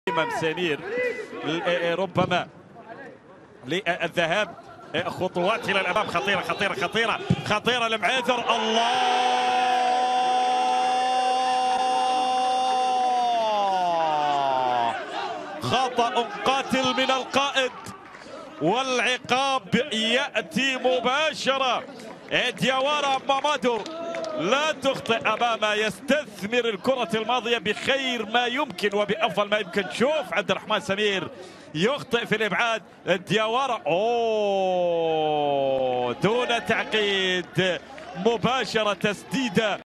Mamadou DIAWARA Goal vs mesameir
Mamadou Diawara football professional player
Club : MUAITHER SC
ÂGE : 30 years